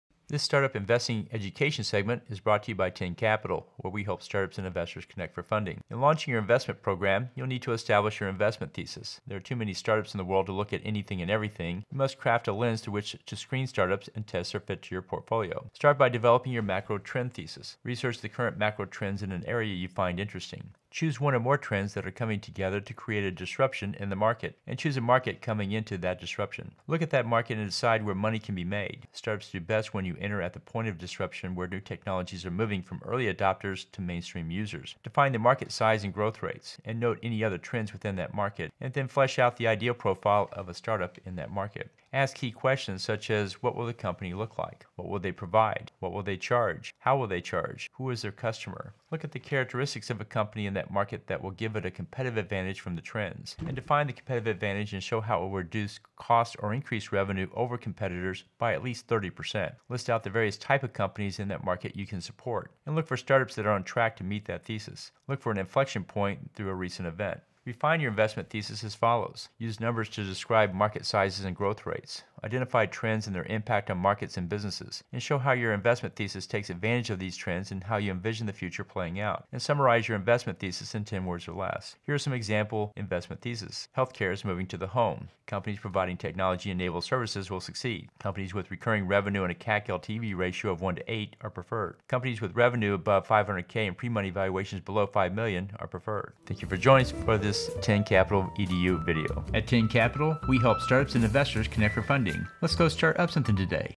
Building Your Investment Thesis
In launching your investment program you’ll need to establish your investment thesis. There are too many startups in the world to look at anything and everything. You must craft a lens through which to screen startups and test their fit to your portfolio.